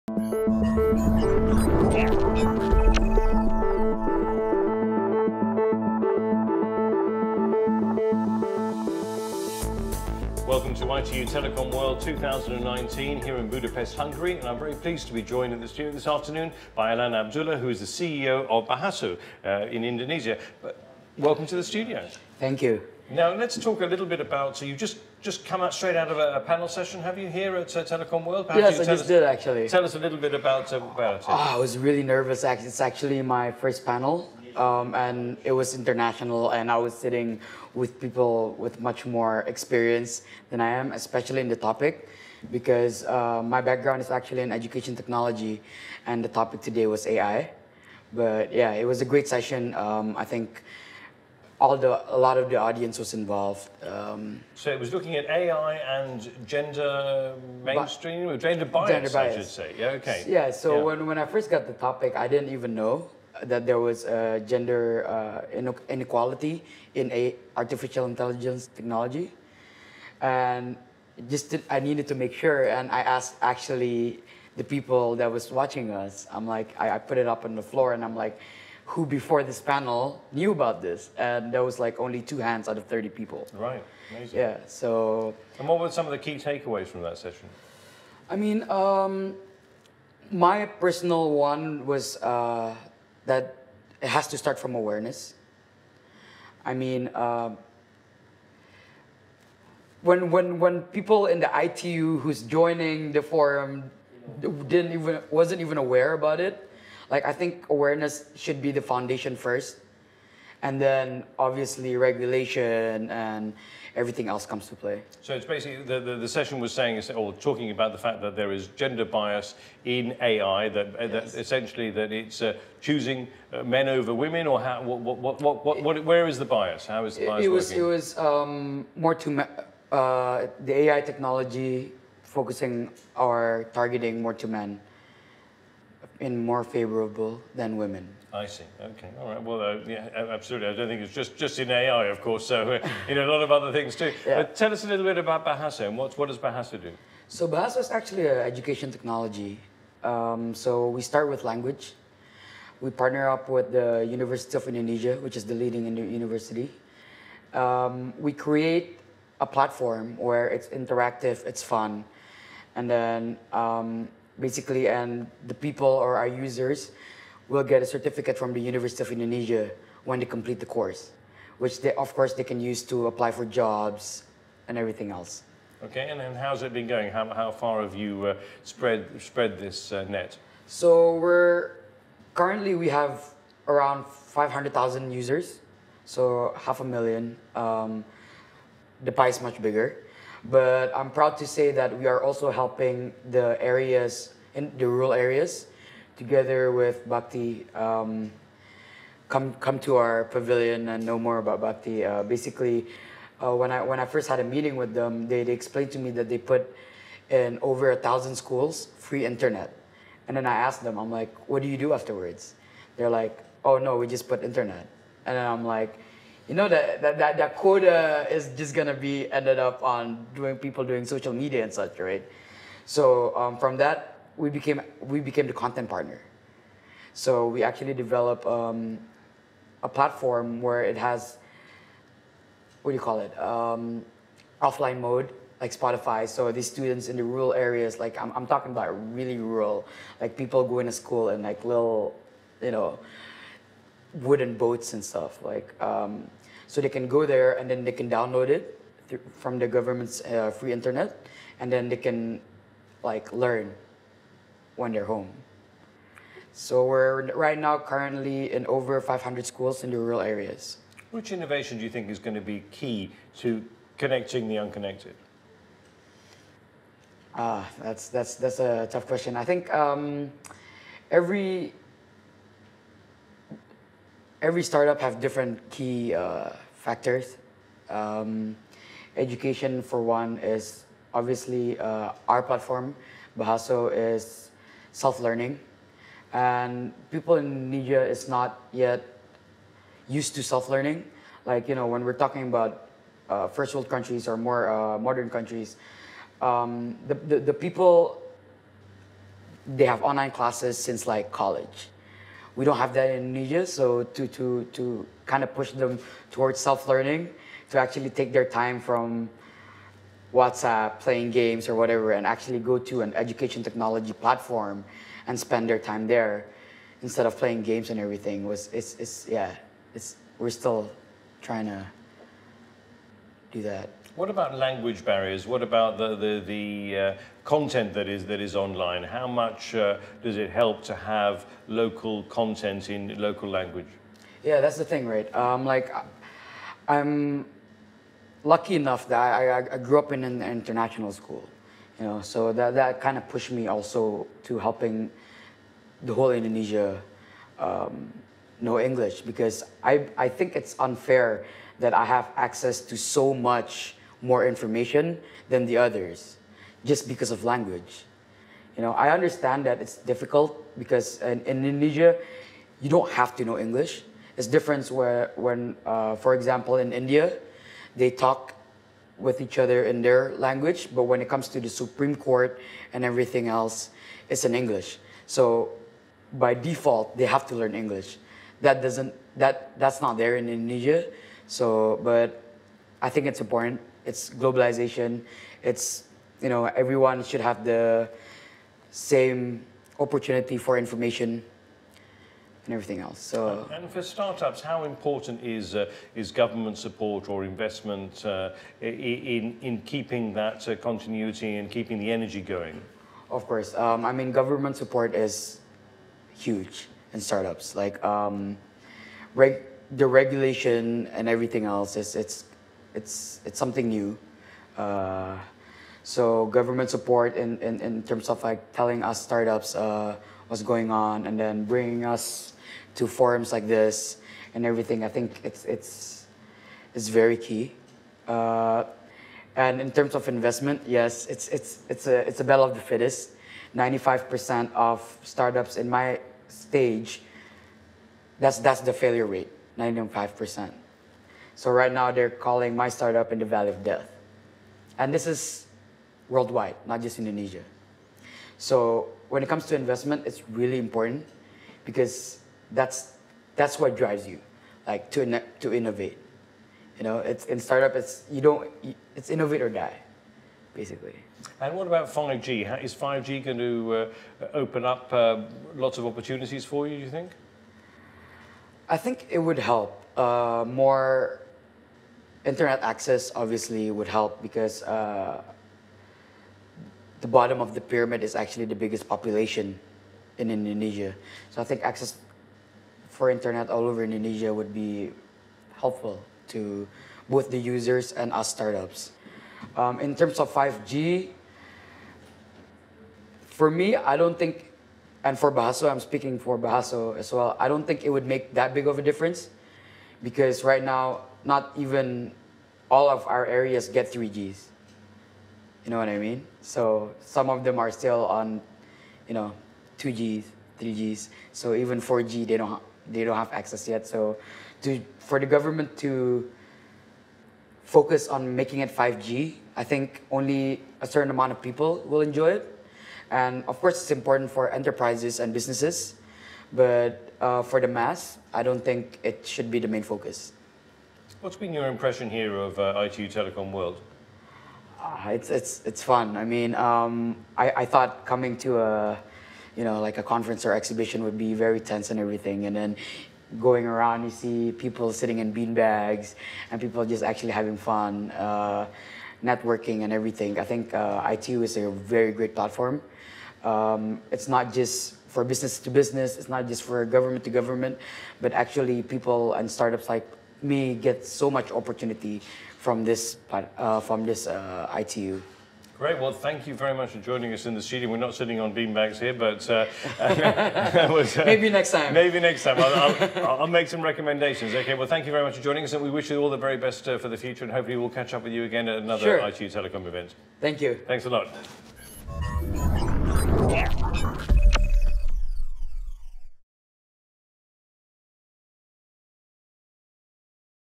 ITU TELECOM WORLD 2019: Allana Abdullah, CEO, Bahaso
Interview with Allana Abdullah, CEO, Bahaso at ITU TELECOM WORLD 2019, Budapest, Hungary 9 - 12 September 2019.